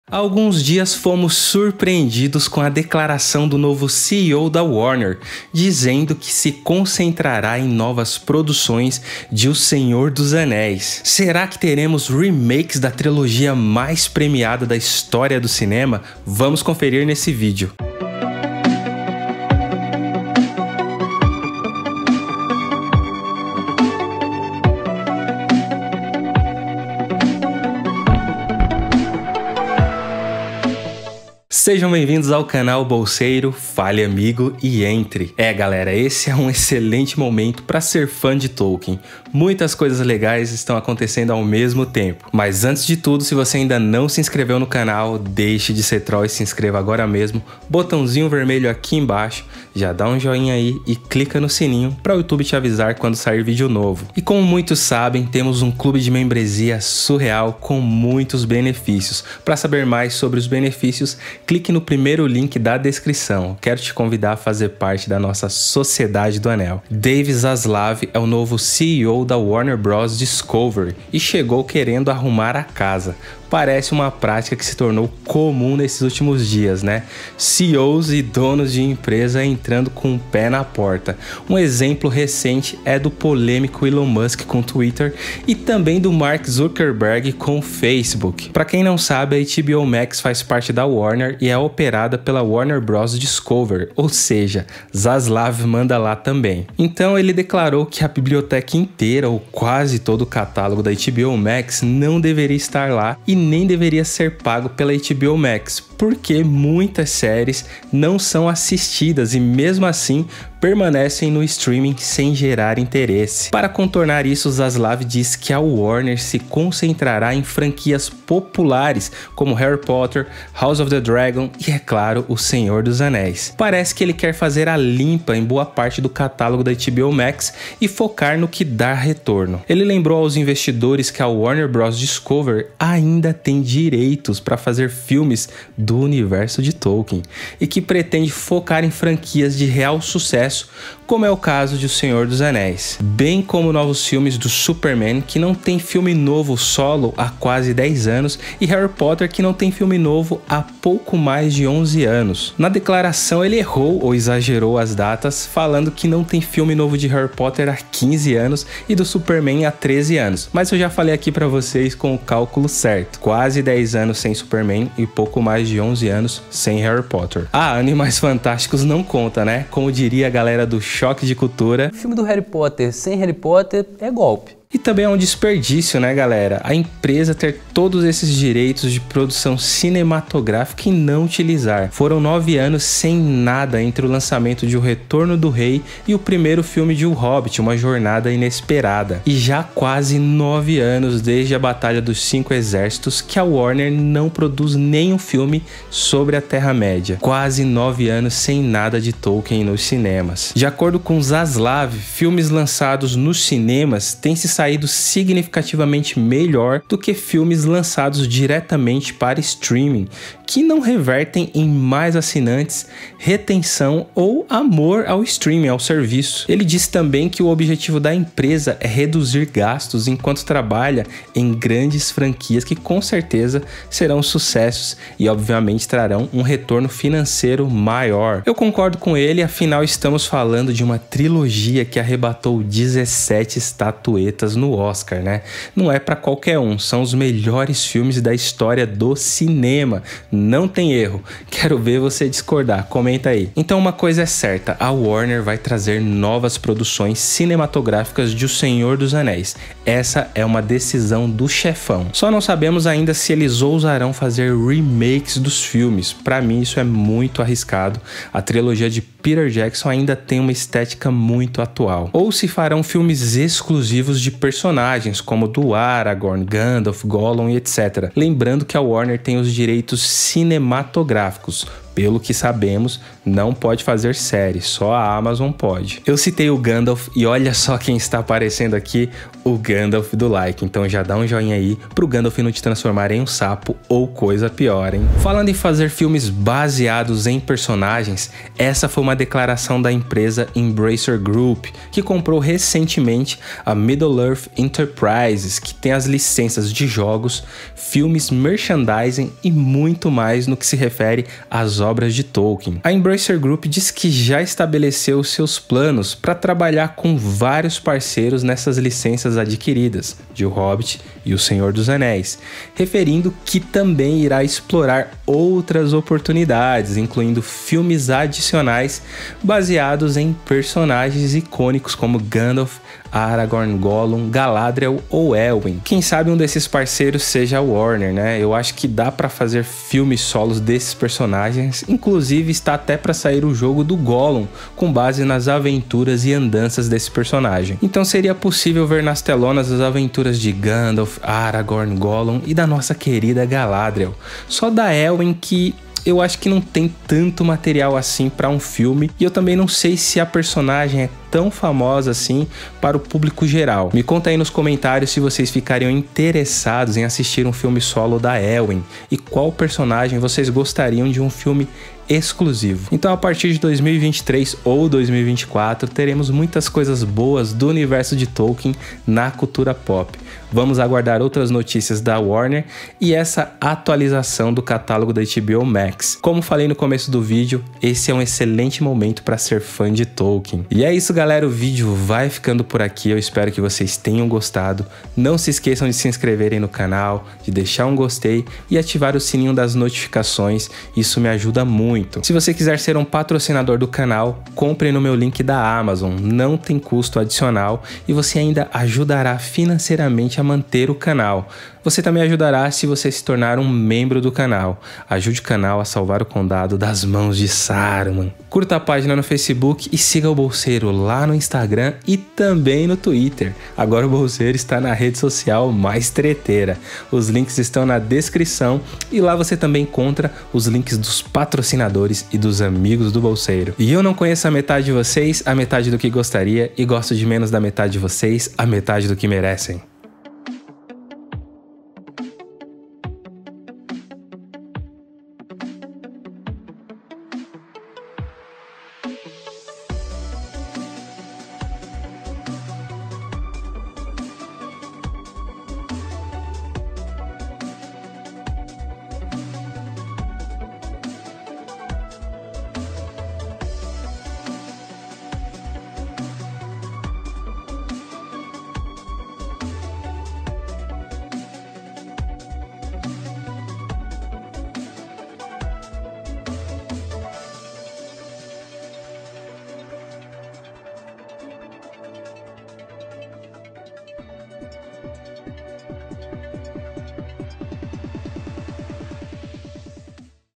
BOMBA! O SENHOR DOS ANÉIS VAI VOLTAR!
Há alguns dias fomos surpreendidos com a declaração do novo CEO da Warner, dizendo que se concentrará em novas produções de "O Senhor dos Anéis". Será que teremos remakes da trilogia mais premiada da história do cinema? Vamos conferir isso nesse vídeo!

⚜️𝐀𝐌𝐈𝐆𝐎𝐒
➤ O Hobbit & O Senhor dos Anéis

J.R.R. Tolkien